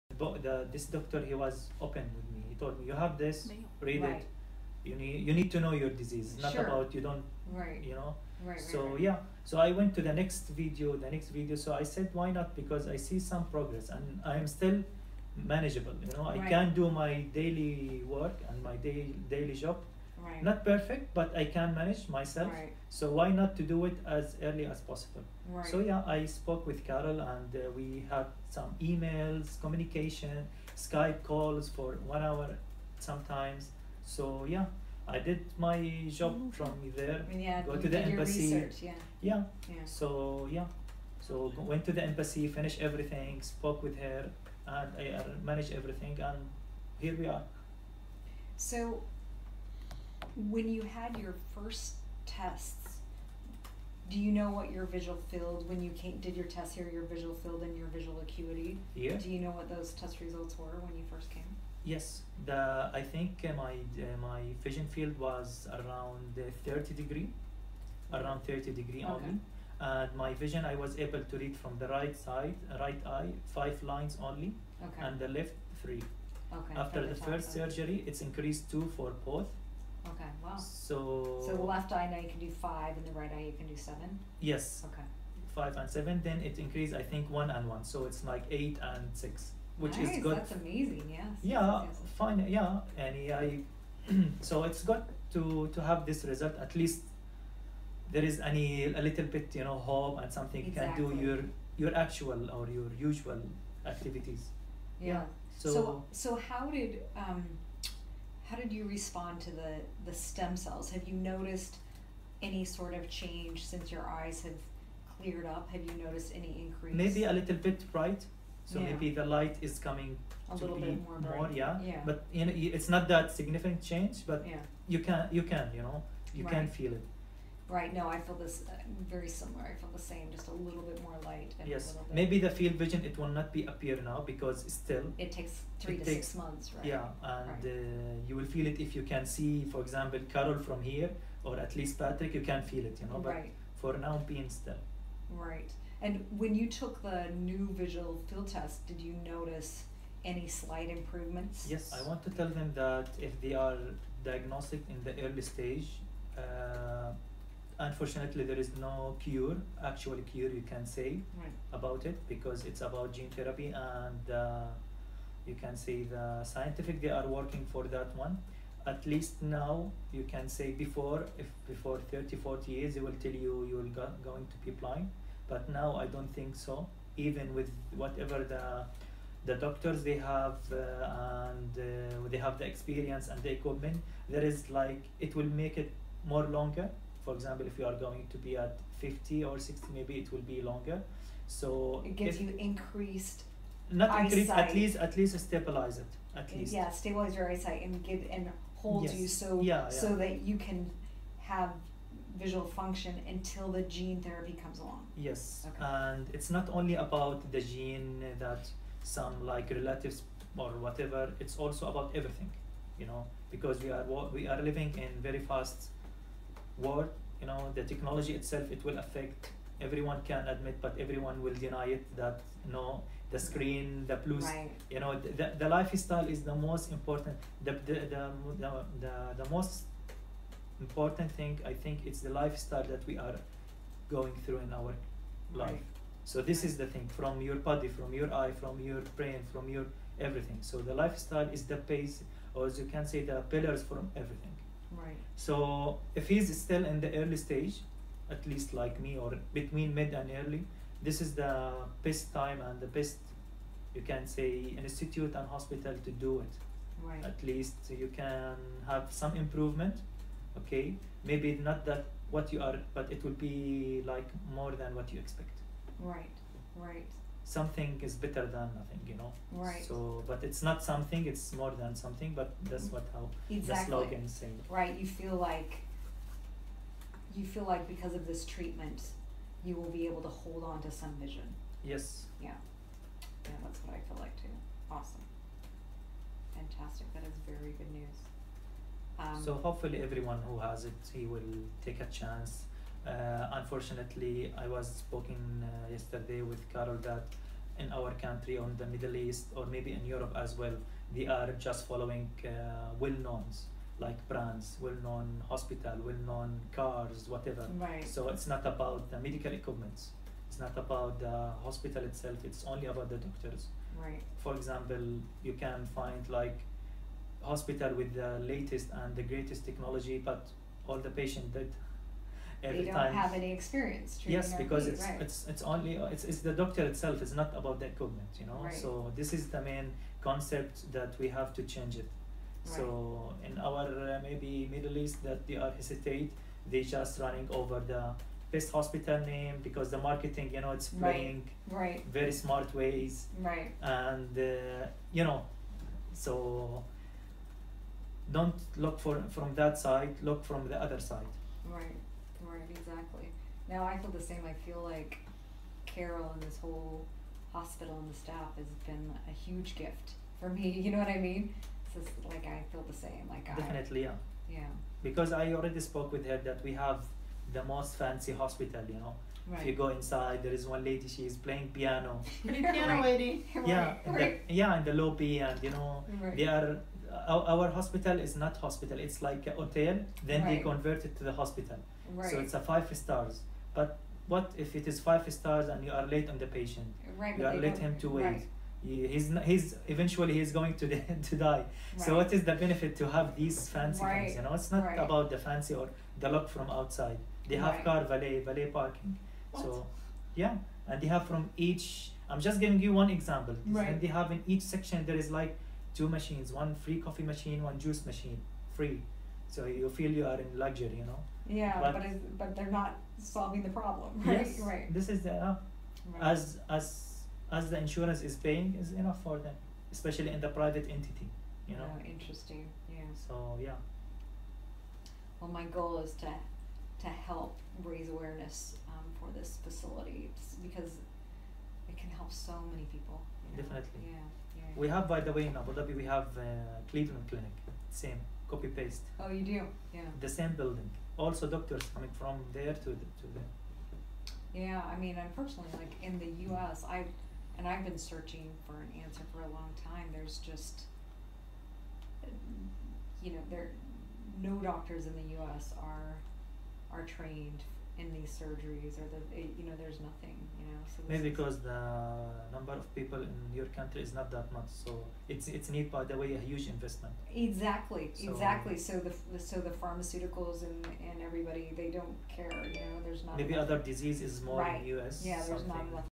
Arabian patient's experence at China RP integrative treatment center
Retinitis Pigmentosa (RP) patient from UAE shares his positive experience at China RP Integrative Treatment Center.